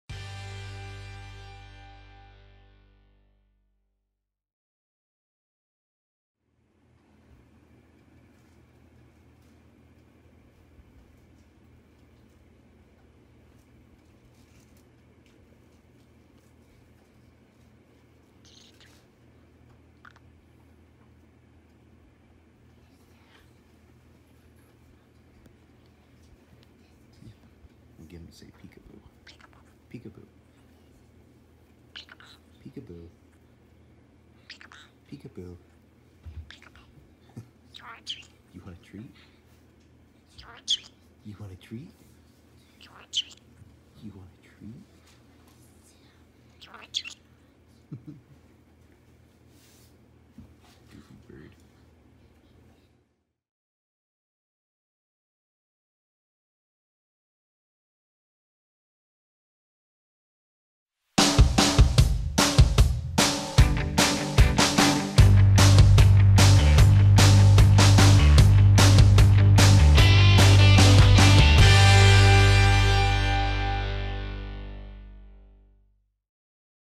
Trello the Green Cheek Conure Talking
I finally got Trello to talk for you.